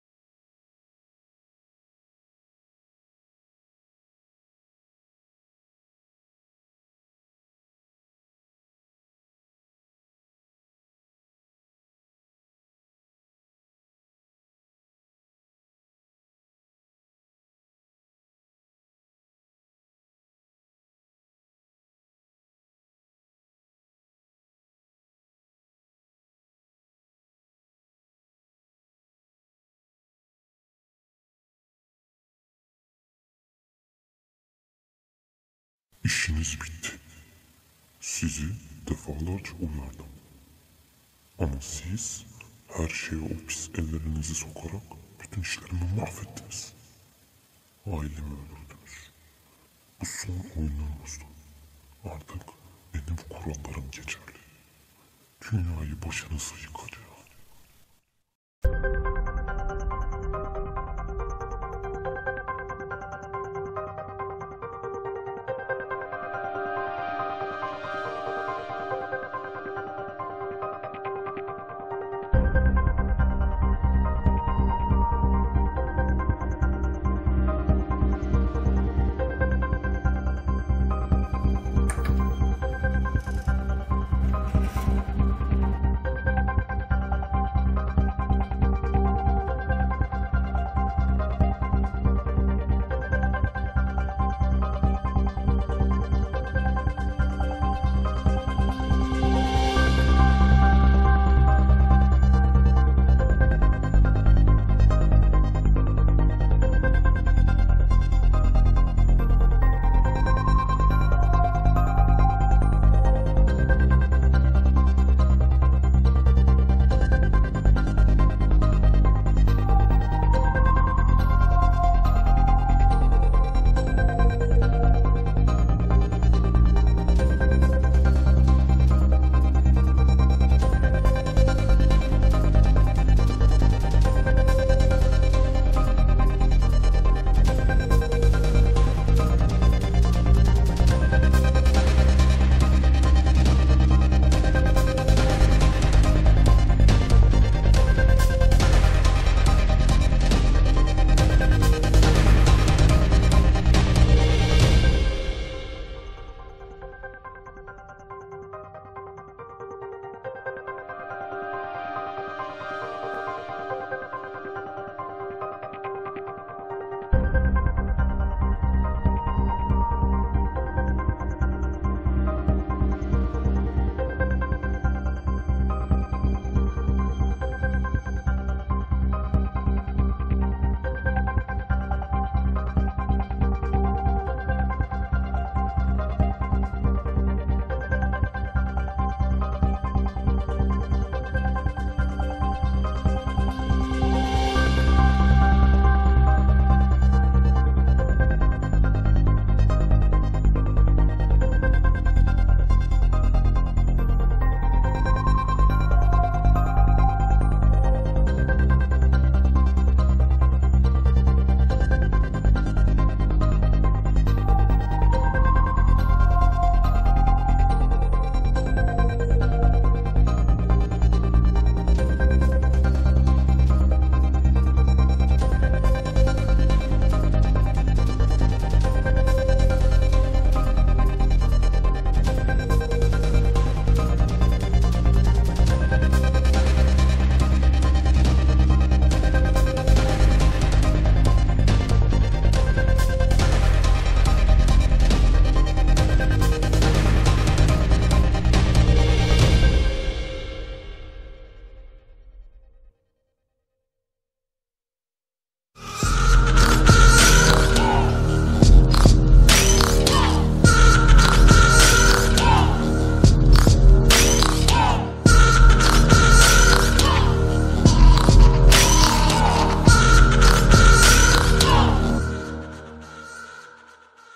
System Down 1.bölüm (sessiz sinema) Anla ve Çöz!
Farklı bir tarz oluşturarak konuşarak değil görüntülerle anlamanızı sağlayacak kısa bir film yaptım.
1. bölüm de mike çeteye network üzerinden büyük bir baskı yapıyor ve çembere alıyor her yerde ifşa olan çete bir yolunu bulup kaçıyor.. ya da kaçtığını zannediyor her şey 2. bölümde belli olacak.

Mike intikamcı ruha sahip bir insan hem teknoloji  hemde fiziksel yönüyle kendini geliştirmiş bir suikast adamı..

Bugüne kadar hep iyilik için savaşan mike ailesini öldüren ve çok kişinin canına ve malına kıymış çetenin bütün üyelerinden intikam alma peşinde..

System Down bir Mustafa Çelik filmdir.
Film tamamen hayal ürünüdür,gerçeklikle alakası yoktur.